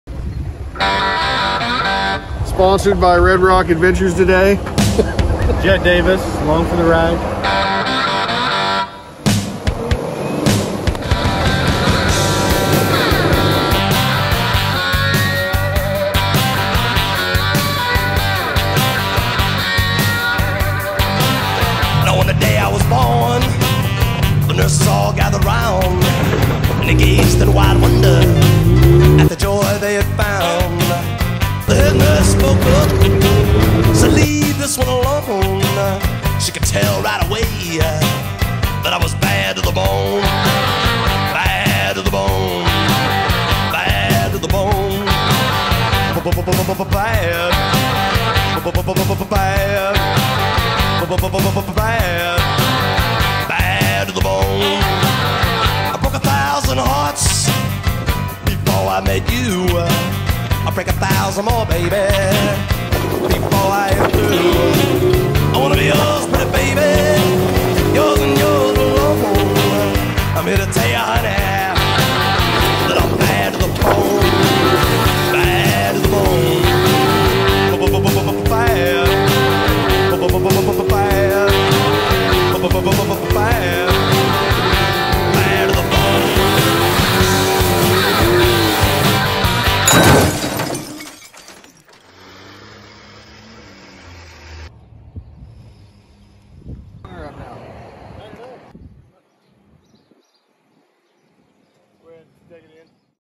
Off Road on the Backway to Crown King Trail, Arizona - No SxS Rentals on a Holiday - a JEEP will DO.
Showing some new friends the back way to Crown King - couldn’t rent a SxS on account of it be’n a holiday weekend and all, but there was a Jeep available, so...

...THANKS Red Rock Adventure Rentals!